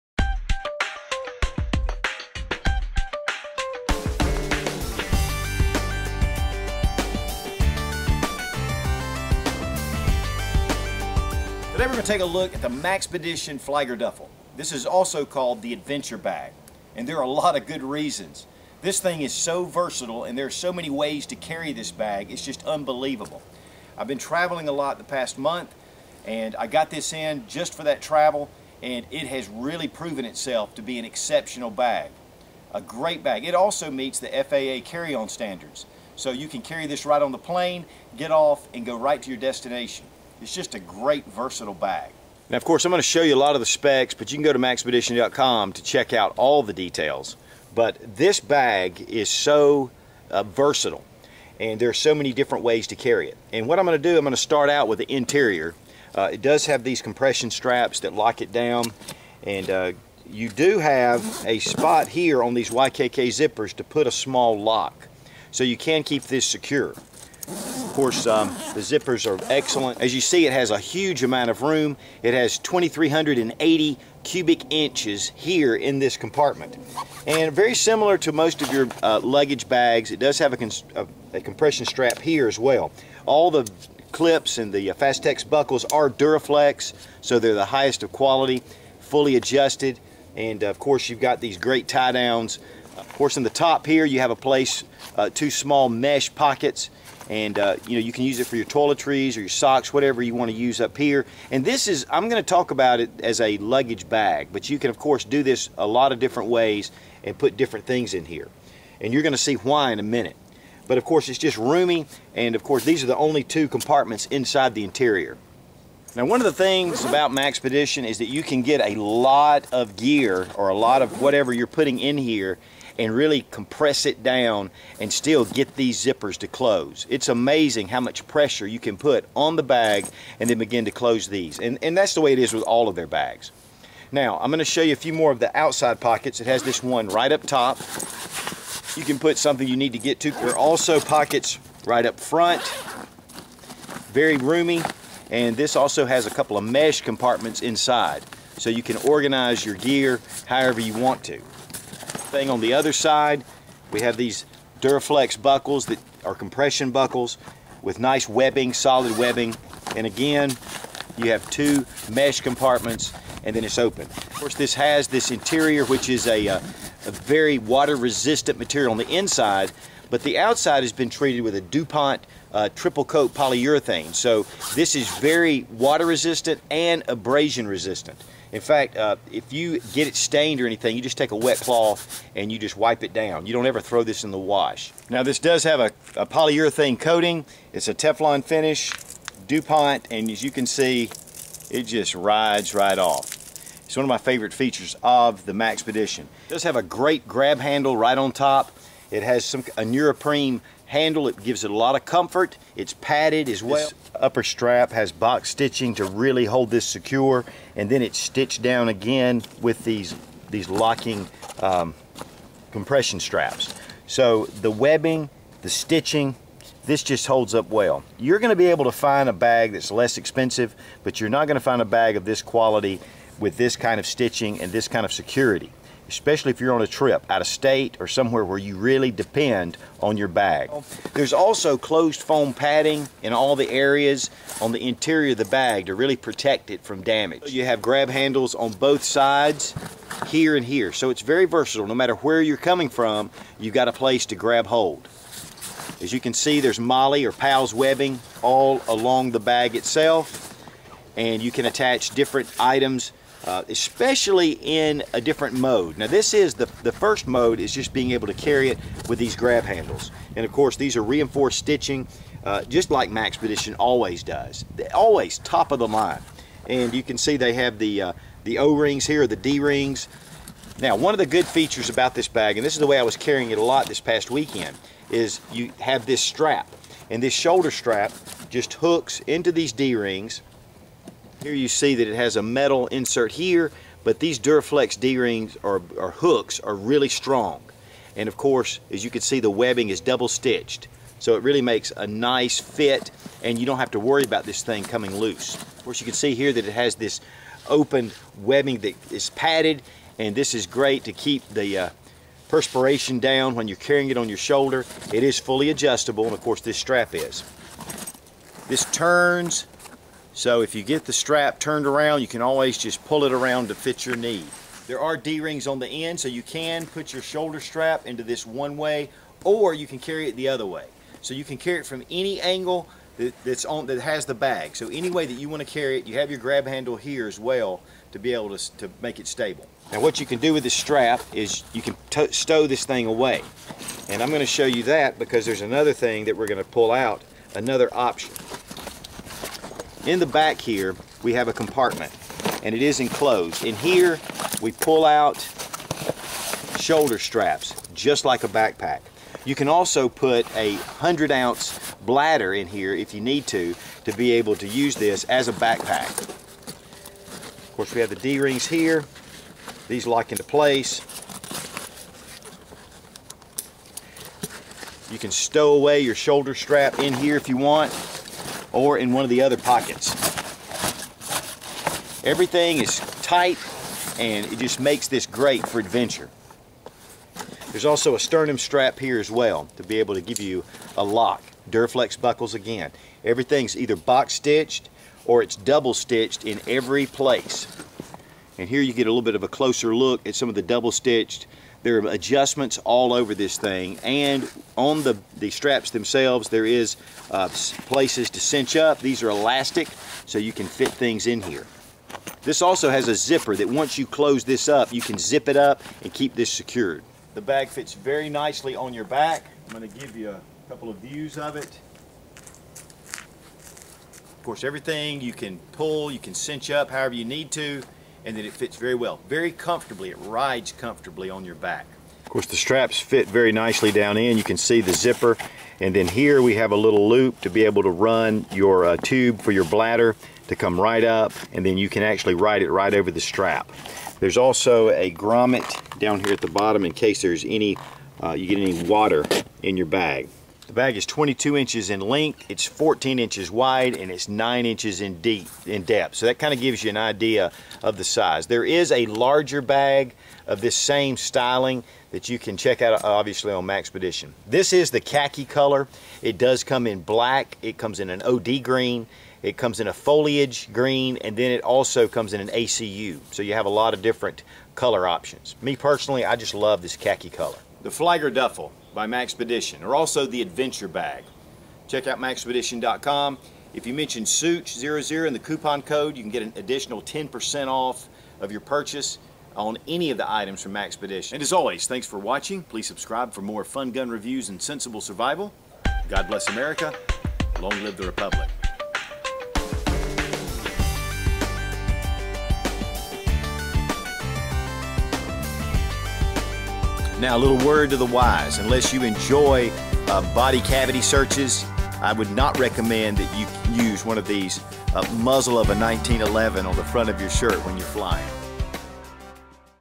Fliegerduffle "Adventure Bag" Maxpedition
Fun Gun Gear Reviews Presents: The Maxpedition Fliegerduffle "Adventure Bag". This can be used as Luggage, Bug Out Bag or Gear Duffle Bag. Very handy and versiltile. Meets FAA Requirements for carry on luggage.